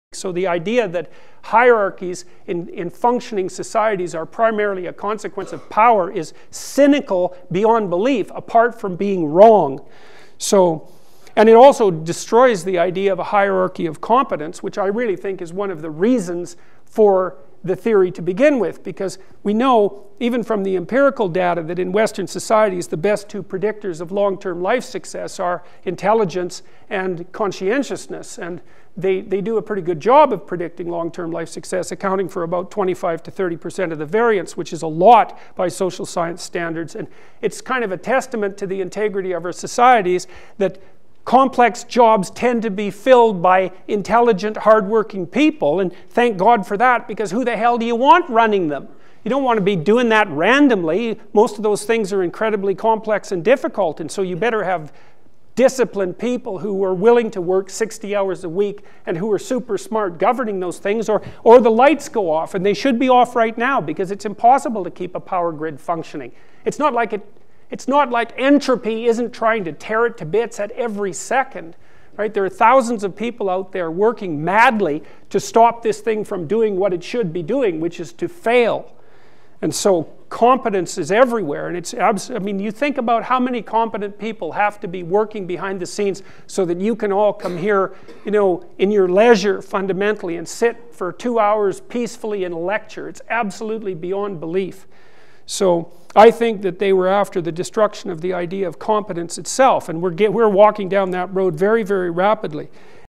Jordan Peterson: Elektrike kiituseks
Kümmekond päeva tagasi Wisconsini Ülikoolis peetud loengus* (minu arvates ühes tema parimatest) üritas doktor Peterson postmodernistidele ja neomarksistidele selgitada, mida elektrivõrk tegelikult tegema peaks.